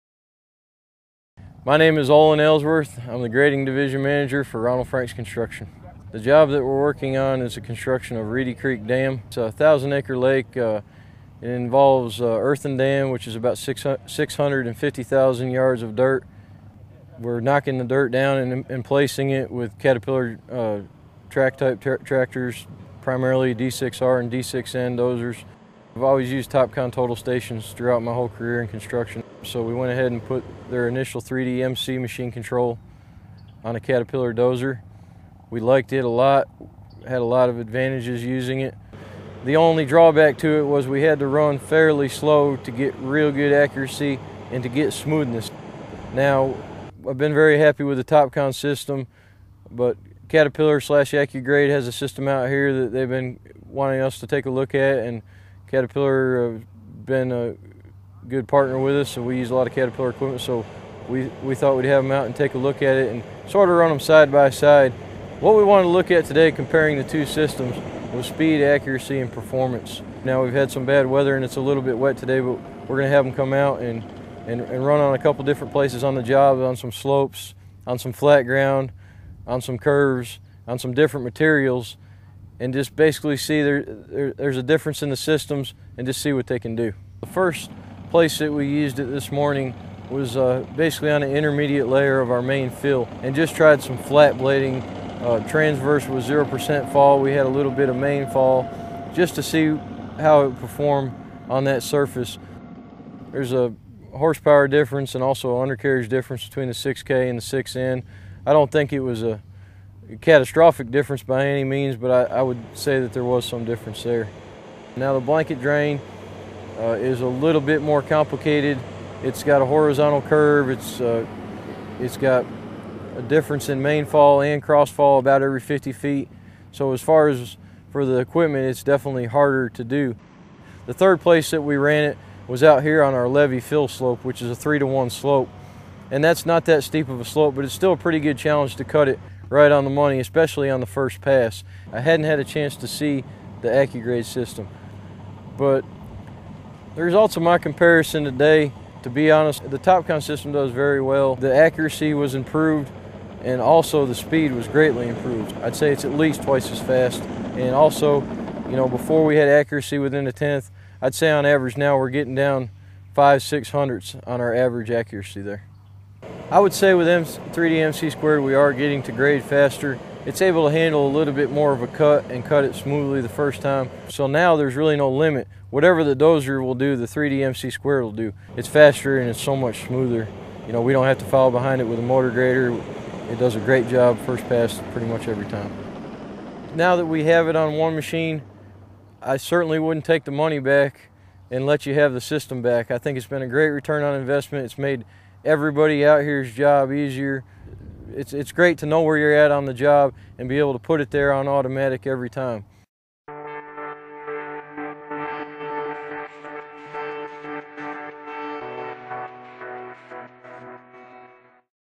Ronald Frank Construction (Jackson, Tenessee) Testimonial
March 2, 2011 07:50 AM |
Olin Ellsworth, Grading Division Manager with Ronald Franks Co... Olin Ellsworth, Grading Division Manager with Ronald Franks Construction discusses using Topcon's 3D-MC2 for their Reedy Creek Dam project.